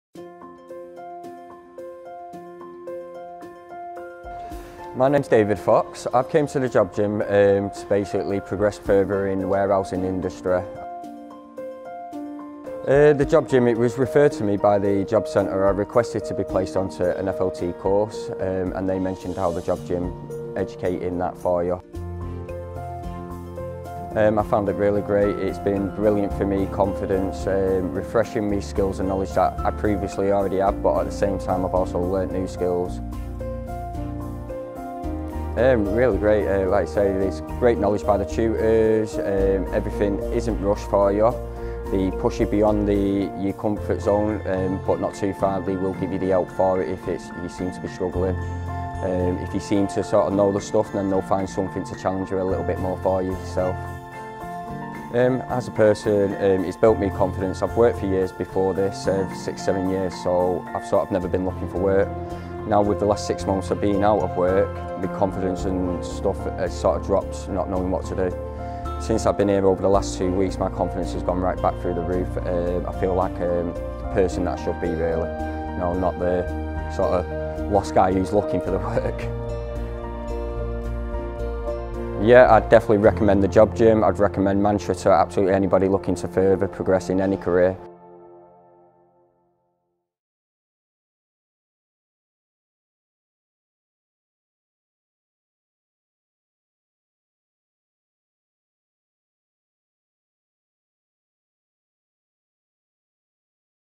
David Fox
Meet David - A past-learner who came to us lacking confidence & the skills to get into work. He has now left us with booming confidence & a job at Best Food.